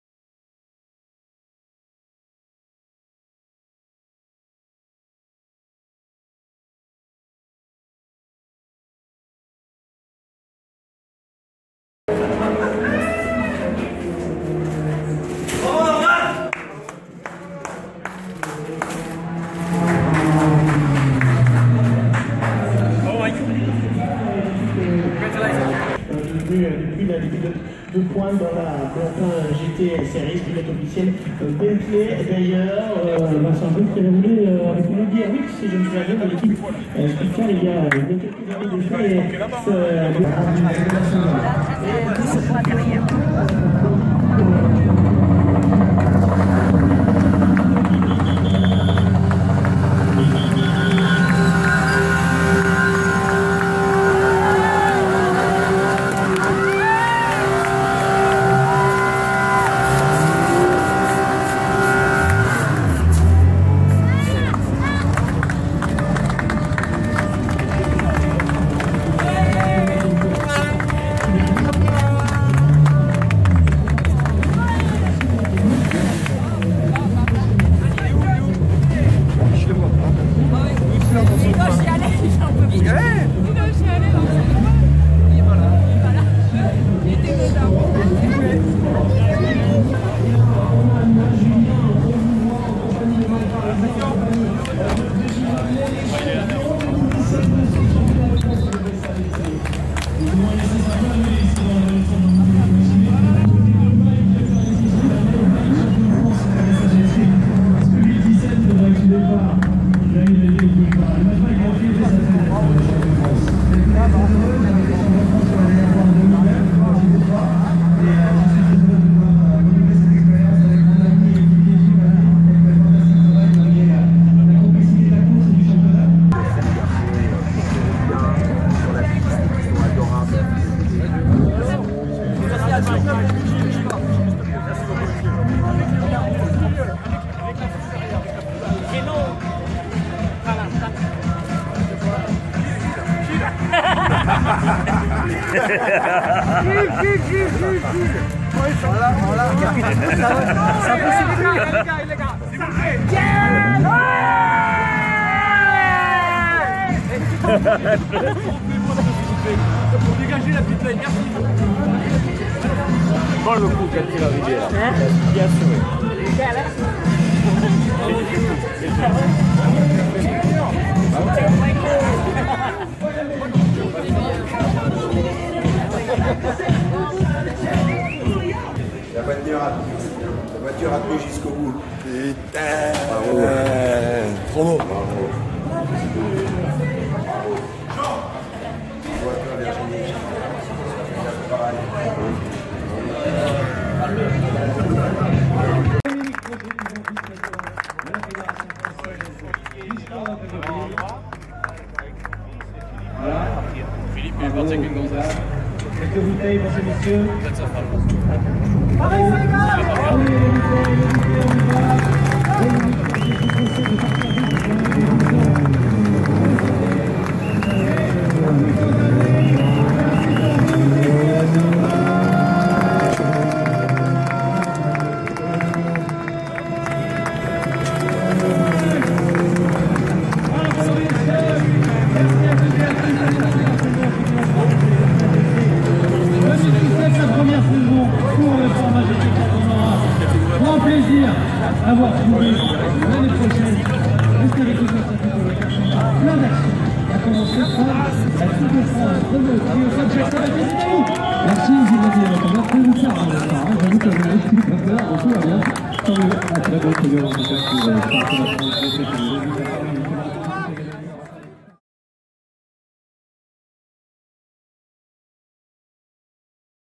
Gilles VANNELET & Mike PARISY Champions de France 2017 FFSA GT4
Petite "Facebook Live" Vidéo réalisée avec un téléphone au Circuit Paul Ricard chez CD Sport.